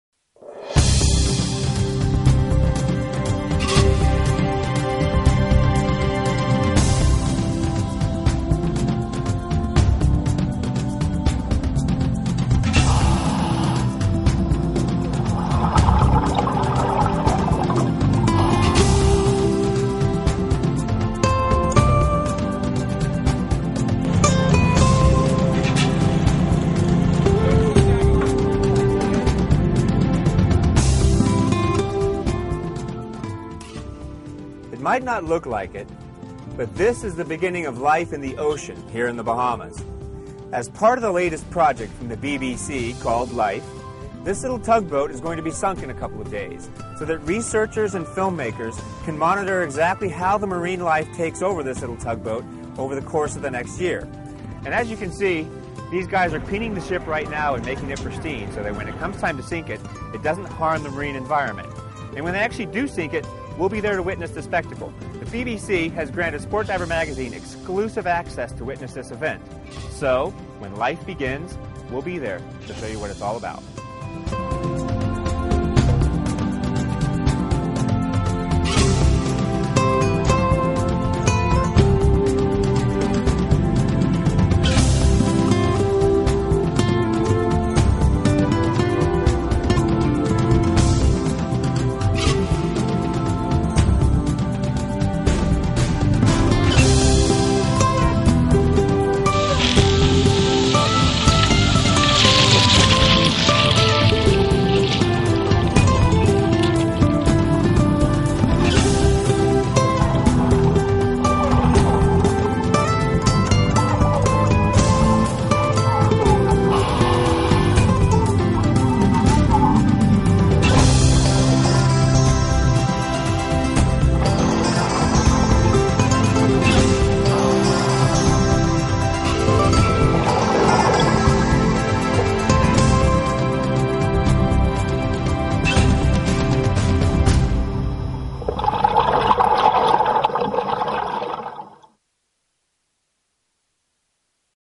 Life
the sinking of a tug boat for an up coming BBC special.